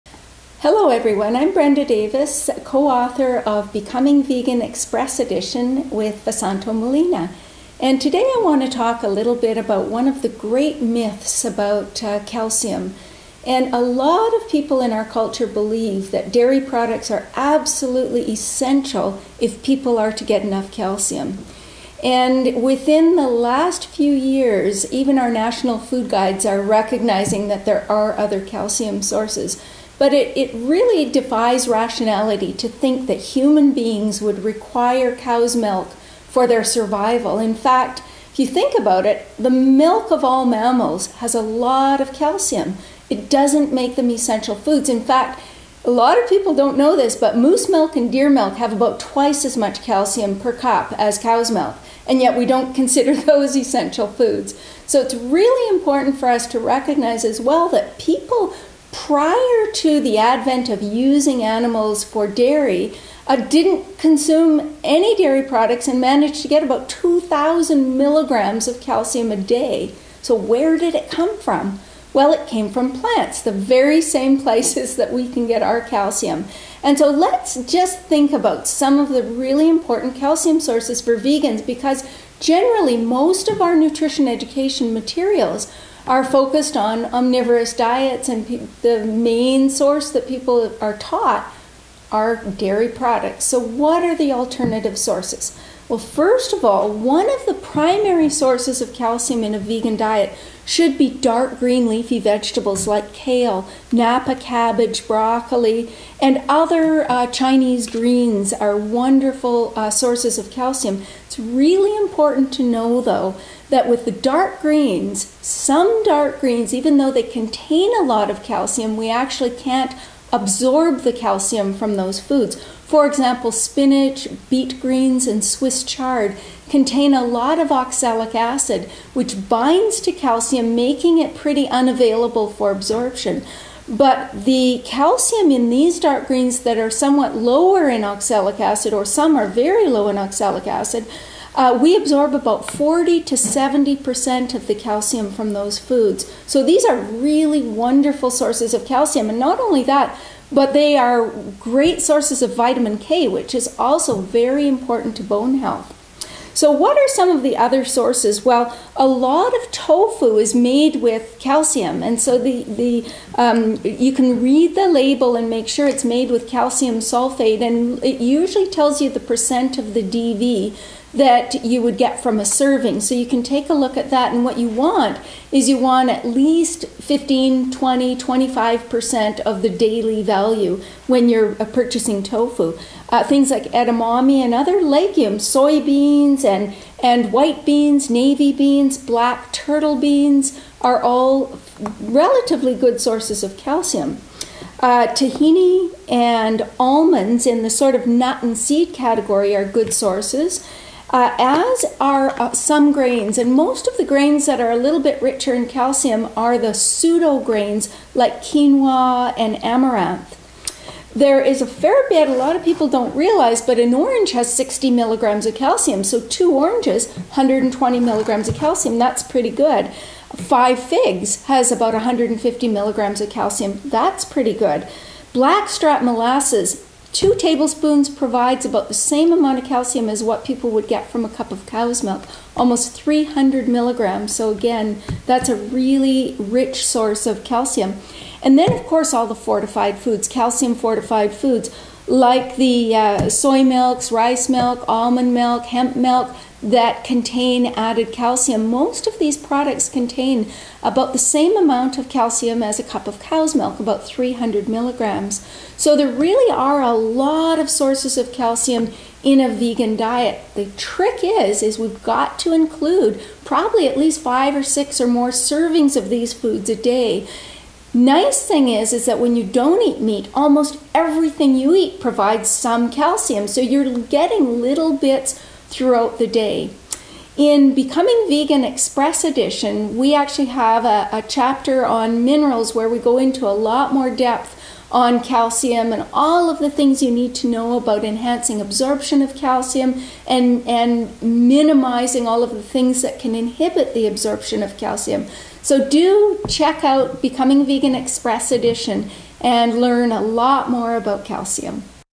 Cow-free Calcium; "Becoming Vegan: Express Edition" Registered Dietitian Brenda Davis
Brenda Davis, Registered Dietitian, co-author of the new "Becoming Vegan: Express Edition" illustrates calcium sources in a plant-based diet and discusses how to meet calcium needs without a drop of cow's milk. Also see "Becoming Raw " Brenda Davis and Vesanto Melina, The Book Publishing Company, 2010. and "Cooking Vegan" by Vesanto Melina and Joseph Forest.  The Book Publishing Company, 2012.